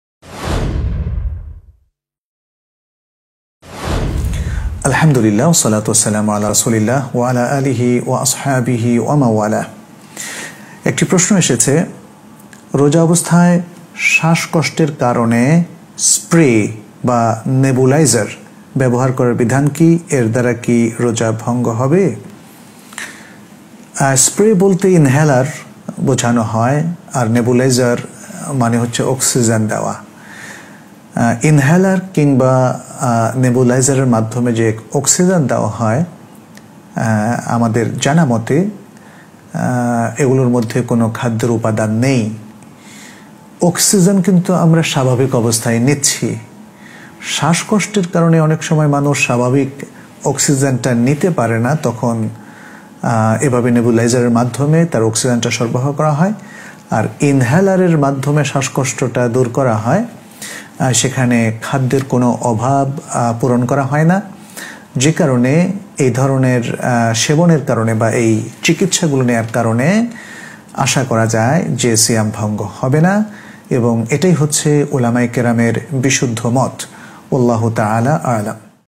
রোজা থাকা অবস্থায় স্প্রে/নেবুলাইজার ব্যবহার করার বিধান কি┇by ডঃ মোহাম্মাদ মঞ্জুর-ই-ইলাহী
রোজা থাকা অবস্থায় স্প্রে/নেবুলাইজার ব্যবহার করার বিধান কি┇

🎙ডঃ মোহাম্মাদ মঞ্জুর-ই-ইলাহী

🎙Dr. Mohammad Monzur-E-Elahi

👋 yeamul islam 👋